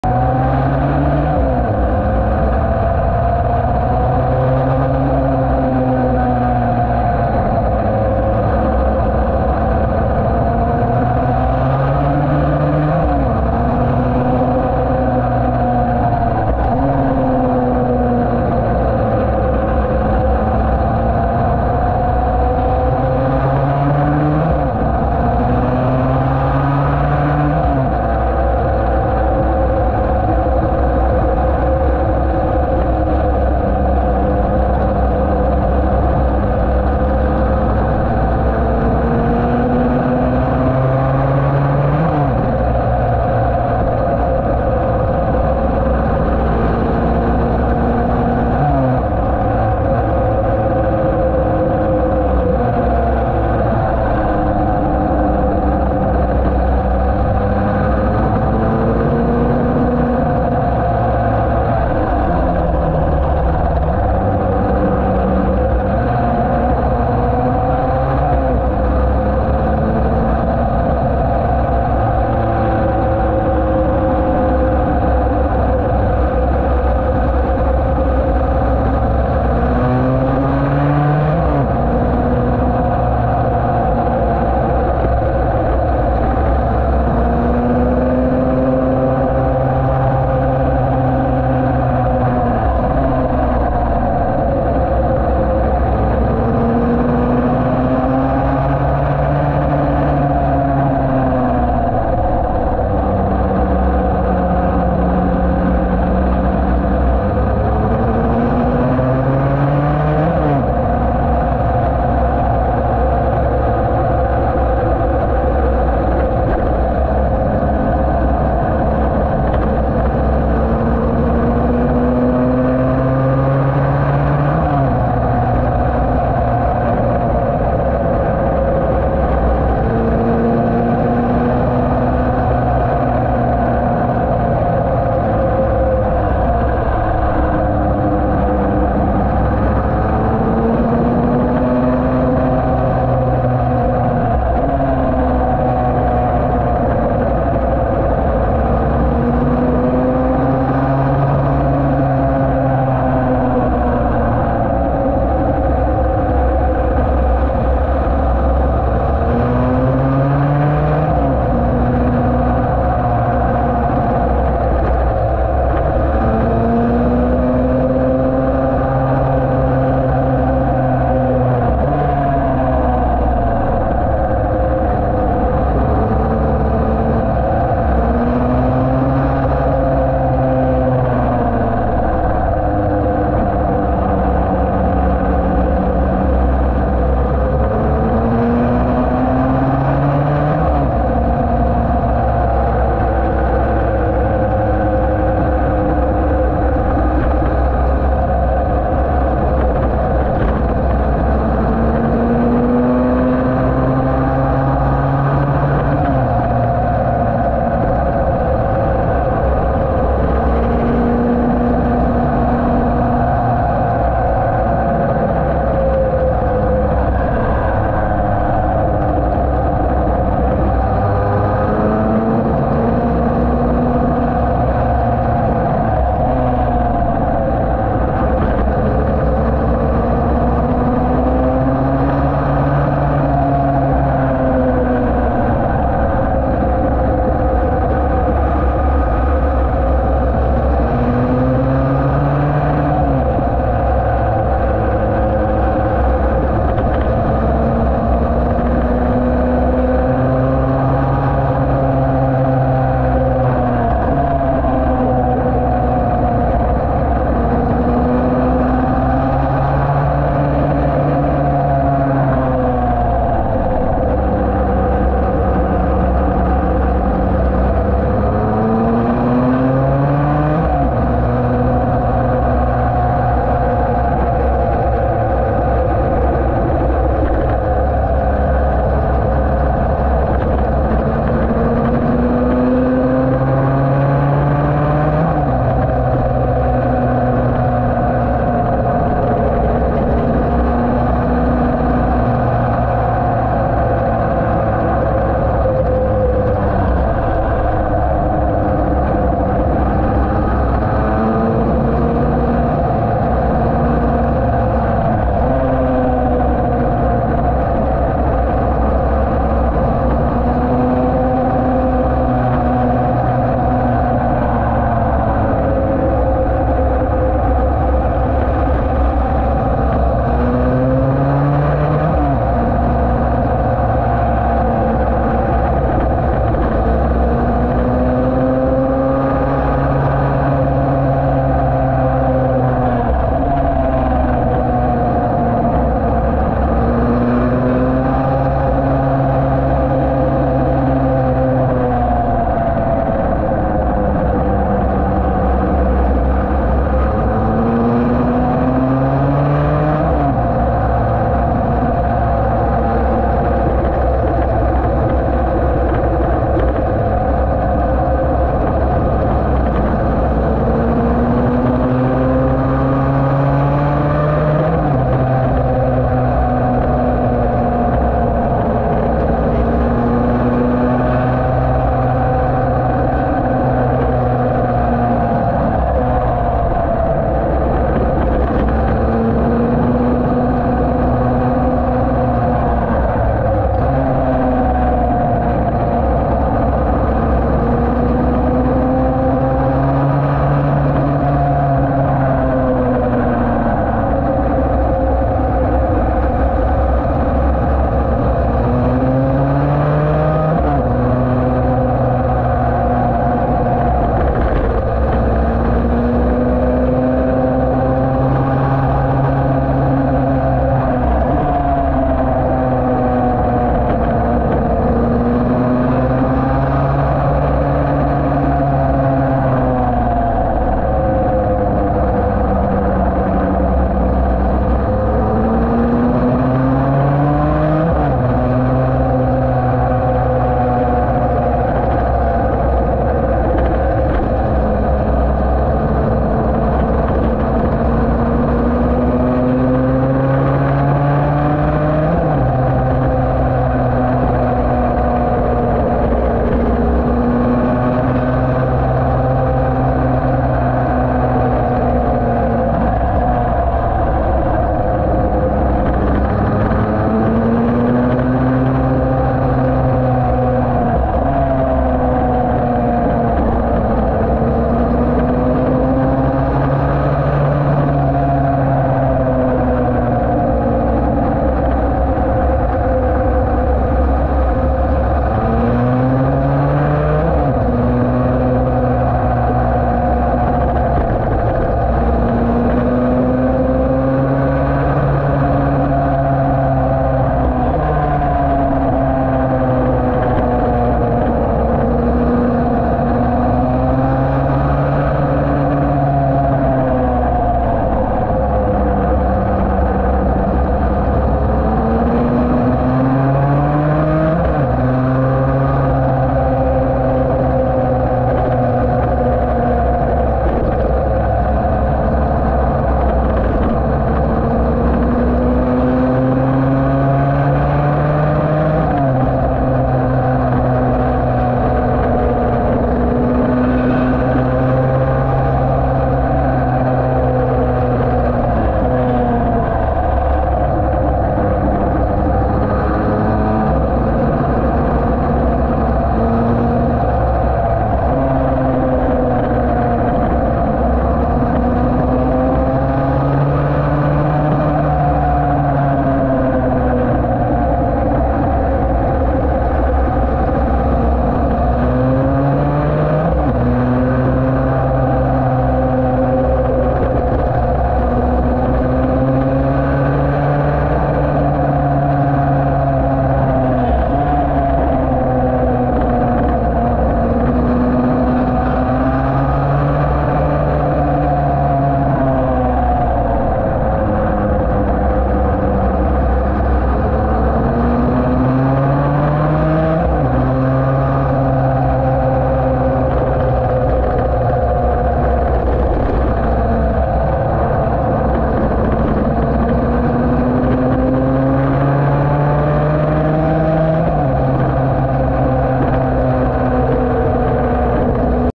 Vintage motorcycle racing, Practice USCRA at Canaan Motorsports Club 8:11:24
Slow (old bike) practice on our Yamaha TD1B Mongrel